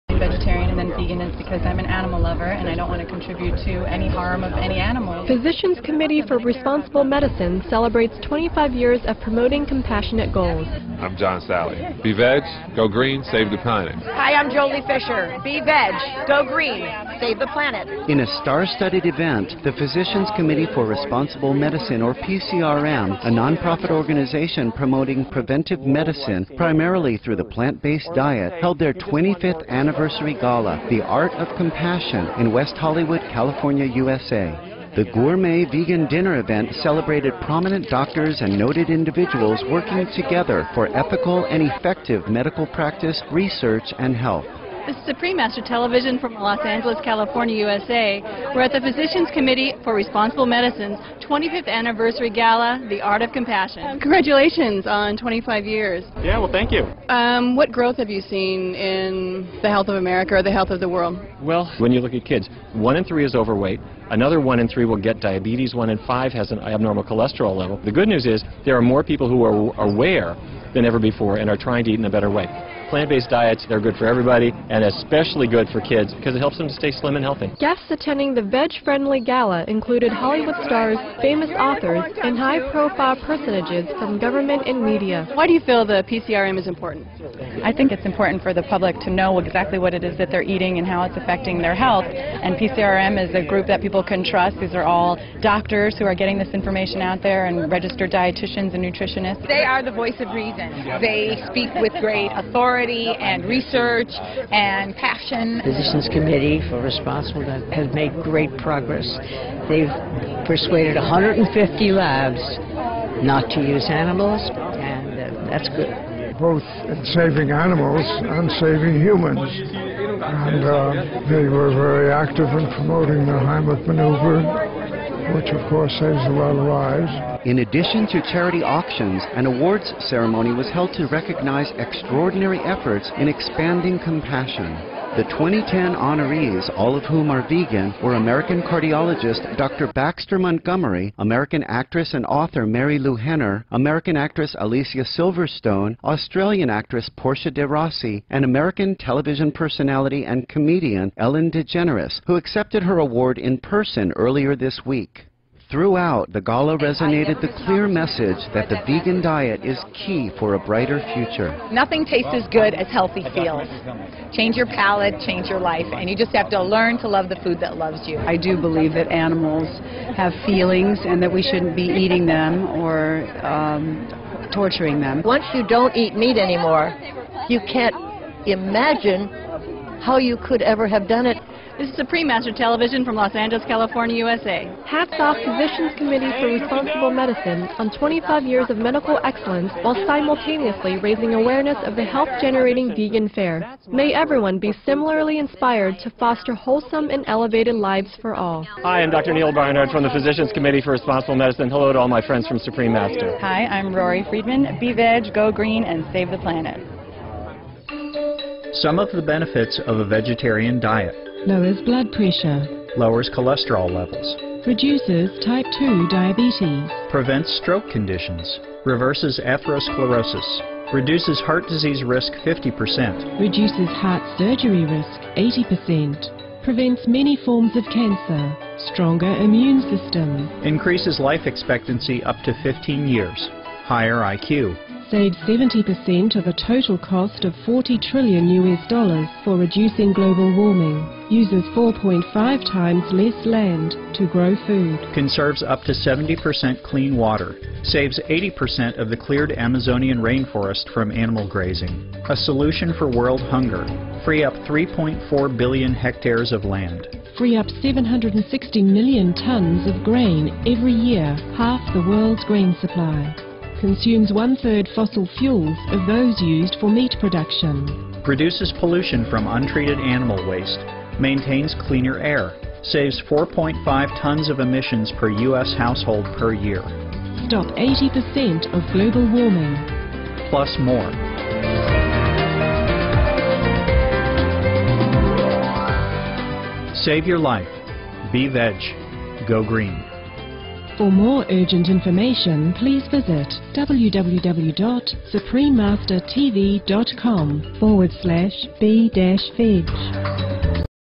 PCRM's Celebrity Green Carpet Gala: Be Healthy Be VEG - SMCH
John Salley - 4-time champion US National Basketball Association (NBA), Vegan (M): Im John Salley. Be veg, go green, save the planet.

Joely Fisher Golden Globe-nominated actress, Vegan (F): Hi, I'm Joely Fisher. Be veg, go green, save the planet.

VOICE: In a star-studded event, the Physicians Committee for Responsible Medicine (PCRM), a nonprofit organization promoting preventive medicine primarily through the plant-based diet, held their 25th Anniversary Gala, The Art of Compassion, in West Hollywood, California, USA.

The gourmet vegan dinner event celebrated prominent doctors and noted individuals working together for ethical and effective medical practice, research and health

Neal Barnard, M.D. Founder and President, Physicians Committee for Responsible Medicine (PCRM), USA, Vegan (M): Yes, well, thank you.

Supreme Master TV (F): What growth have you seen in the health of America or health of the world?

Neal Barnard, M.D. (M): When you look at kids, one in three is overweight. Another one in three will get diabetes; one in five has an abnormal cholesterol level.

The good news is there are more people who are aware than ever before and are trying to eat in a better way. Plant-based diets are good for everybody and especially good for kids because it helps them stay slim and healthy.

VOICE: Guests attending the veg-friendly gala included Hollywood stars, famous authors, and high-profile personages from government and media.

Supreme Master TV (F): Why do you feel PCRM is important?

Rory Friedman Bestselling author on vegan diet, USA, Vegan (F): I think it's important for the public to know exactly what it is that they are eating and how it's affecting their health, and PCRM is a group that people can trust. These are all doctors who are getting this information out there, and registered dietitians and nutritionists.

Marilu Henner - Actress and author of Total Health Makeover, 2010 Voice of Compassion Award, Vegan (F): They are the voice of reason. They speak with great authority and research and passion.

Peggy McCay - Primetime Emmy Award-winning actress (F): Physicians Committee for Responsible Medicine has made great progress. They've persuaded 150 labs not to use animals and that's good.

Dr. Henry Heimlich - American physician, inventor of Heimlich maneuver (M): Its both saving animals and saving humans. And they were very active in promoting the Heimlich Maneuver, which of course saves a lot of lives.

VOICE: In addition to charity auctions, an awards ceremony was held to recognize extraordinary efforts in expanding compassion. The 2010 honorees, all of whom are vegan, were American cardiologist Dr. Baxter Montgomery; American actress and author Marilu Henner; American actress Alicia Silverstone; Australian actress Portia de Rossi, and American television personality and comedian Ellen DeGeneres, who accepted her award in person earlier this week.

Throughout, the gala resonated the clear message that the vegan diet is key for a brighter future.

Marilu Henner (F): Nothing tastes as good as healthy feels. Change your palate, change your life and you just have to learn to love the food that loves you.

Francis Fisher Hollywood actress, Titanic, Vegan (F): I do believe that animals have feelings and that we shouldn't be eating them or torturing them.

Cloris Leachman - Academy Award-winning actress, Vegetarian (F): Once you dont eat meat anymore, you cant imagine how you could ever have done it.

VOICE: Hats off Physicians Committee for Responsible Medicine, on 25 years of medical excellence while simultaneously raising awareness of the health-generating vegan fare.  May everyone be similarly inspired to foster wholesome and elevated lives for all.

Neal Barnard, M.D. (M): Hi, Im Dr. Neal Barnard from Physicians Committee for Responsible Medicine: Hallo to all my friends from Supreme Master TV!

Rory Friedman Bestselling author on vegan diet, USA, Vegan: Hi, I'm Rory Friedman. Be veg, go green and save the planet.

Supreme Master TV (F): This is Supreme Master Television from Los Angeles, California, USA. Were at the Physicians Committee for Responsible Medicine's 25th Anniversary Gala: The Art of Compassion.
Supreme Master TV (F): Congratulations on 25 years!